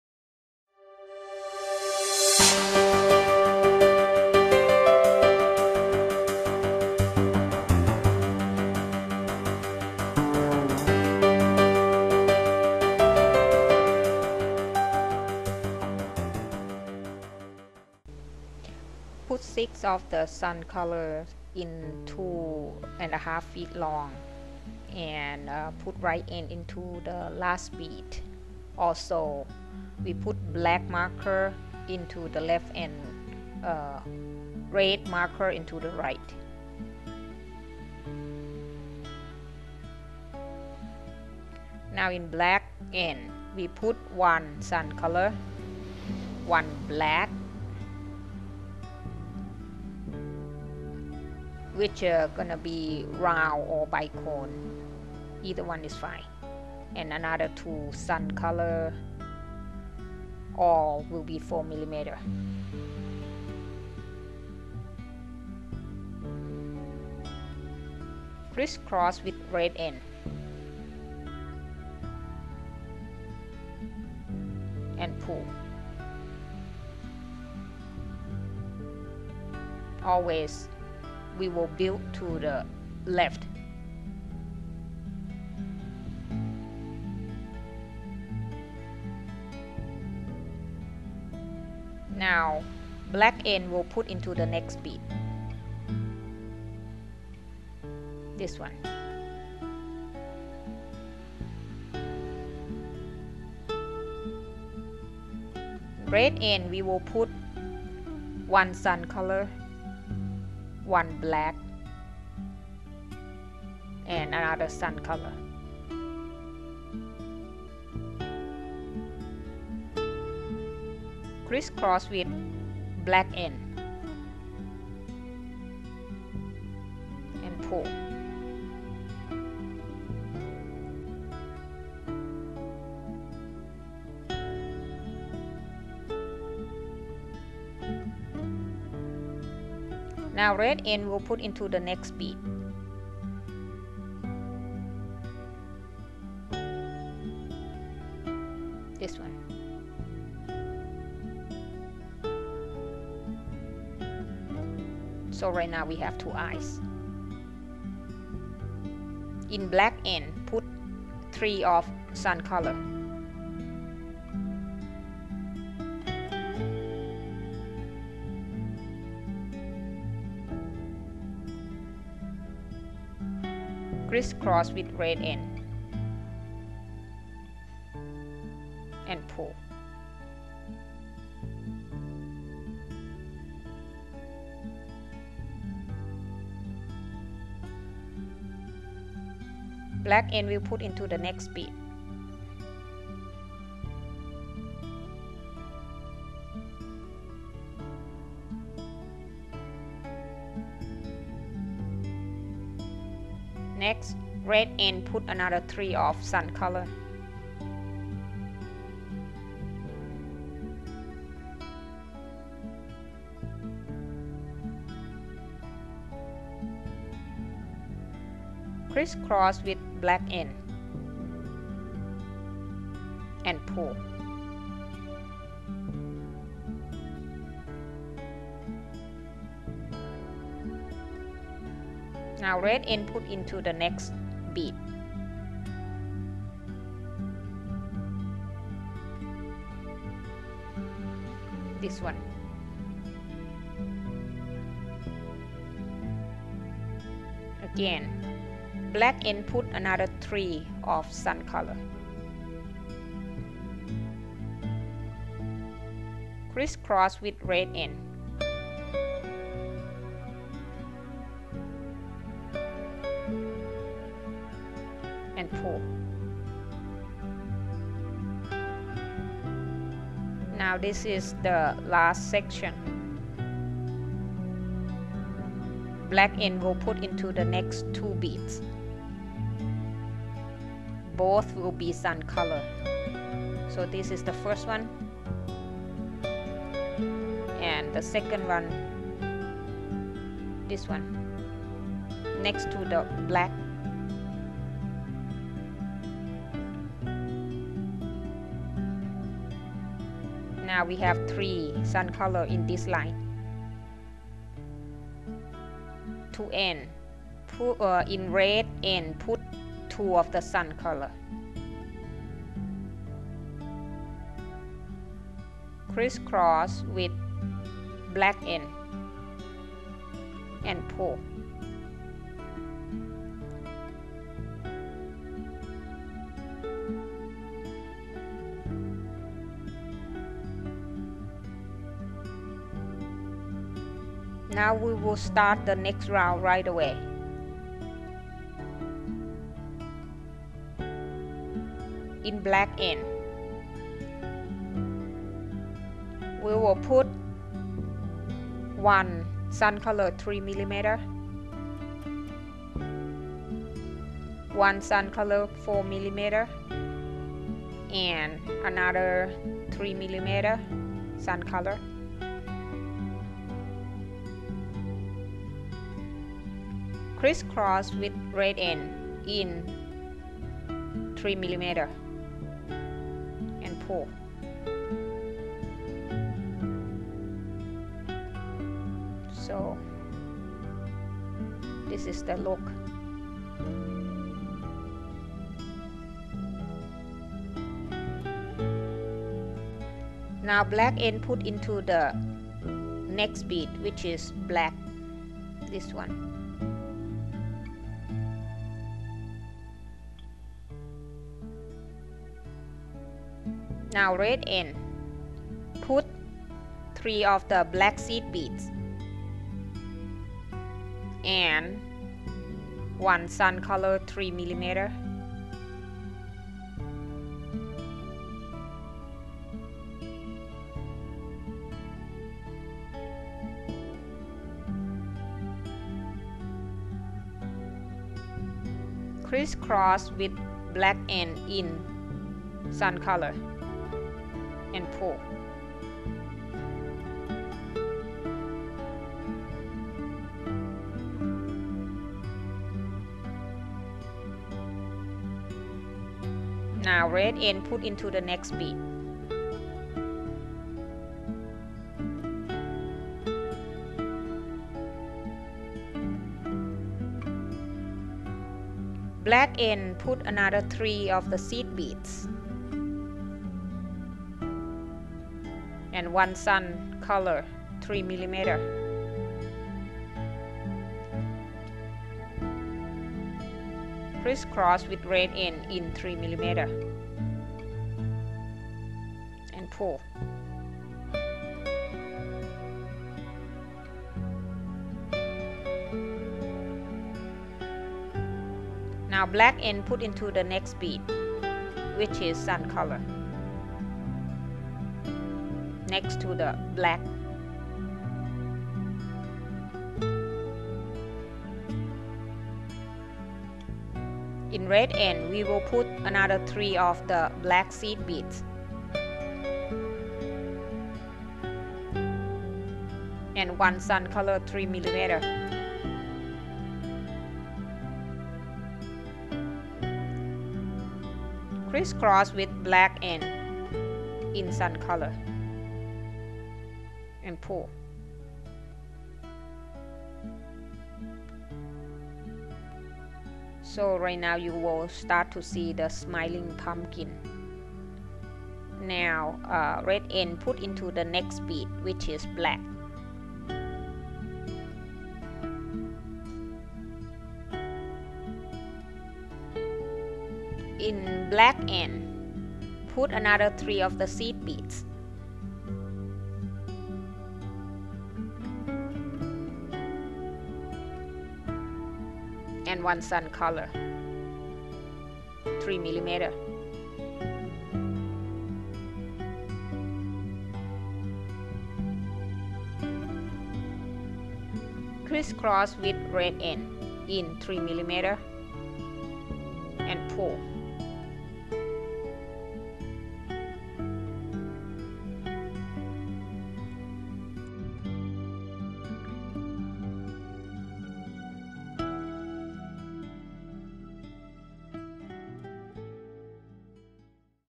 Crystal Pumpkin Head Part 1
This Pumpkin Head is perfect for the Halloween season. It made from Swarovski crystal in Sun color.
Free video tutorials beads animals.